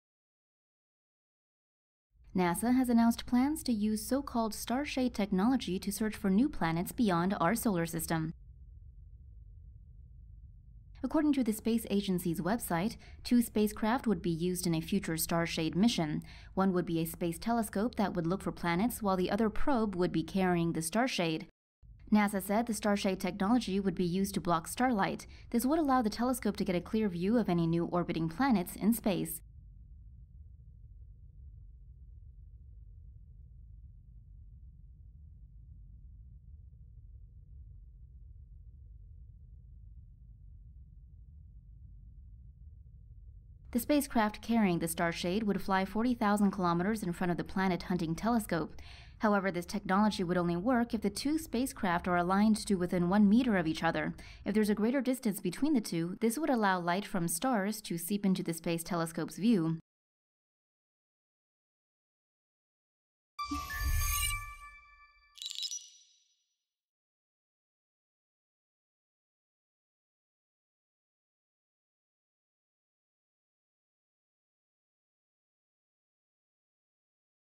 NASA to use "starshade" technology in search for new worlds
NASA has announced plans to use starshade technology to search for new planets beyond our solar system.

Two spacecraft would be used in a future starshade mission, according to the space agency's website.

One would be a space telescope that would look for planets while the other probe would be carrying the starshade.

NASA said the starshade technology would be used to block starlight. This would allow the telescope to get a clear view of any new orbiting planets in space.

The spacecraft carrying the starshade would fly 40,000 kilometers in front of the planet-hunting telescope.

NASA explained that this technology would only work if the two spacecraft stay are aligned to within one meter of each other. If there is a greater distance between the two, this would allow light from star to seep into the space telescope's view.

RUNDOWN SHOWS:
1. The new starshade technology and the solar system
2. The space telescope and the probe carrying the starshade
3. How the starshade would work in space
4. The starshade and the space telescope floating in space

VOICEOVER (in English):
"NASA has announced plans to use so-called starshade technology to search for new planets beyond our solar system."

"According to the space agency's website, two spacecraft would be used in a future starshade mission."

"However, this technology would only work if the two spacecraft stay are aligned to within one meter of each other. If there is a greater distance between the two, this would allow light from star to seep into the space telescope's view."